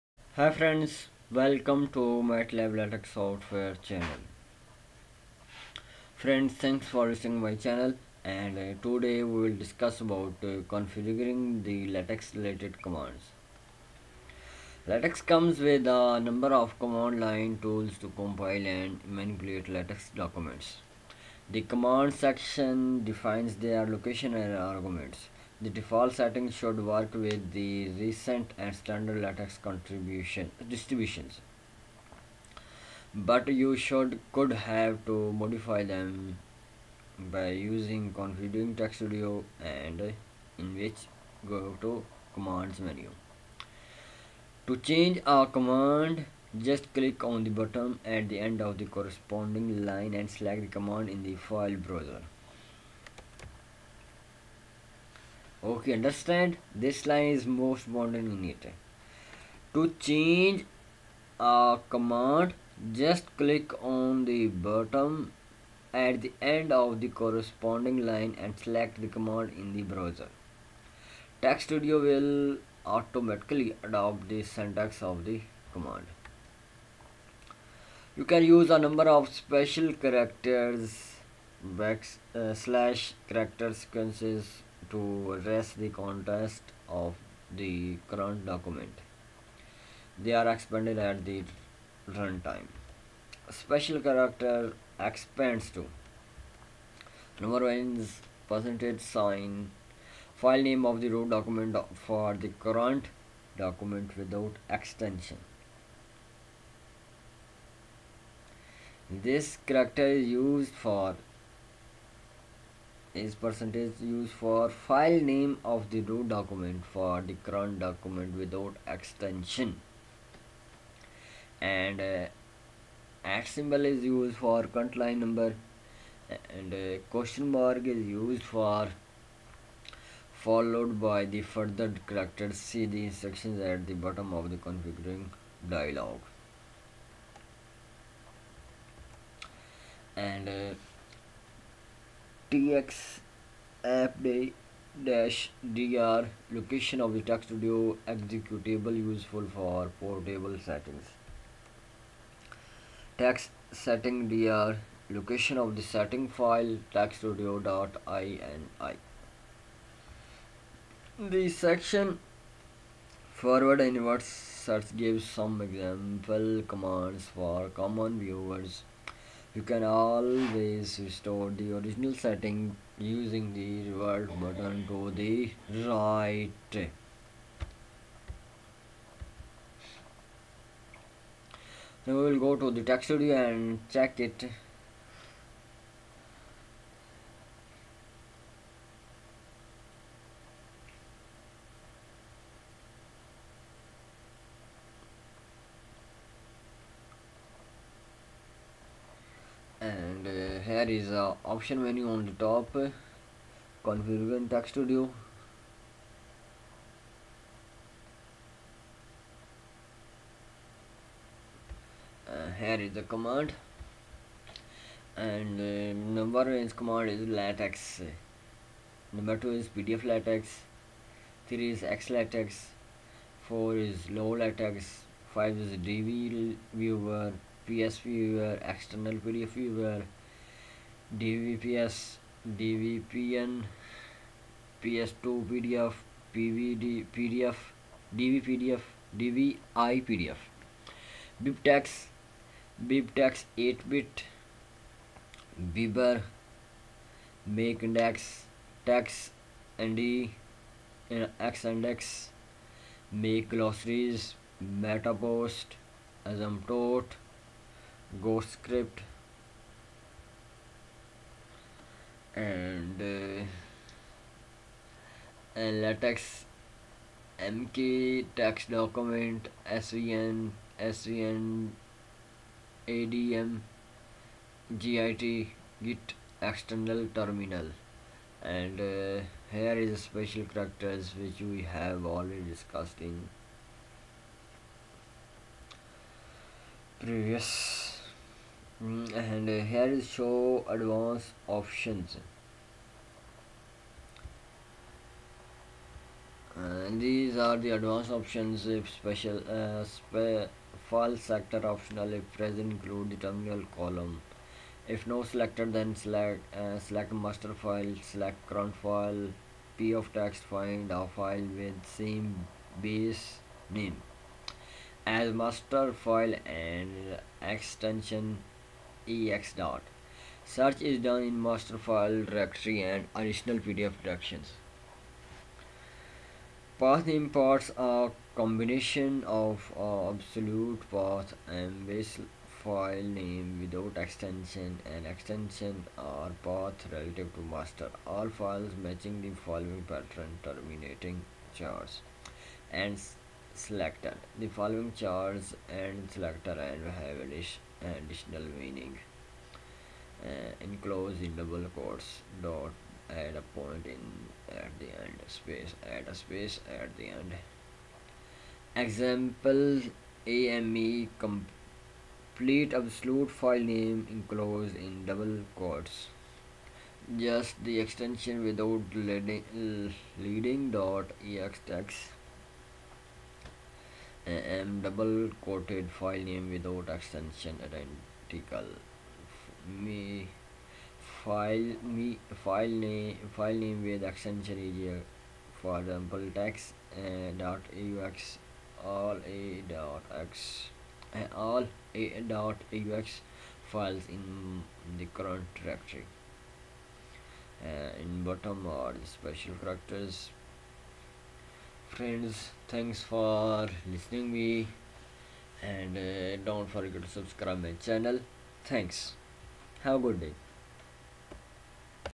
Configuring the latex related commands | Configure TeXstudio
Configuring the latex related commands
LaTeX comes with a number of command line tools to compile and manipulate LaTeX documents. The commands section defines there location and arguments. The default settings should work with the recent and standard LaTeX distributions, but you could have to modify them ("Configure TeXstudio" in "Commands"). To change a command, just click on the button at the end of the corresponding line and select the command in the file browser: TeXstudio will automatically adapt the syntax of the command.